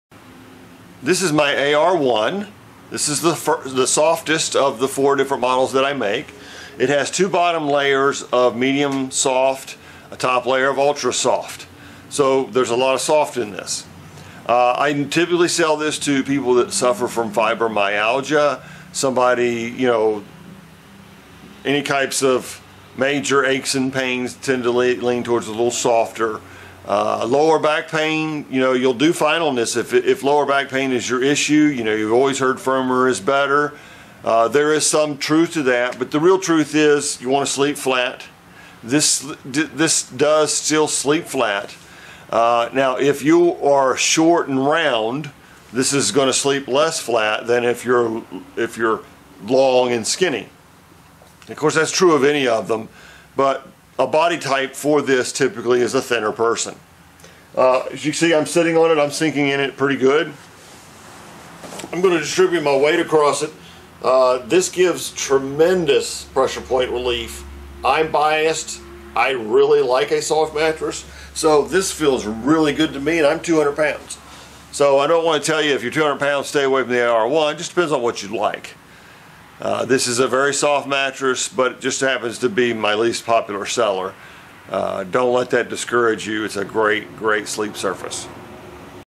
Arrangement Recommendation 1 (AR1) Mattress - Our Softest Model & Most Luxuriously Soft
This is the Arrangement Recommendation 1 (AR1) mattress.  It is our softest model of the four ARs.  If you want to wrap yourself in soft comfort, this is the model for you.  With two bottom layers of medium soft and a top layer of ultra soft, it is really soft while still being supportive.  Some want to go even softer with perhaps a medium layer of soft instead of medium soft but we think you still need that support layer beneath the ultra soft top layer.  Admittedly, this is our lest popular model, it is still great for small people who need softness.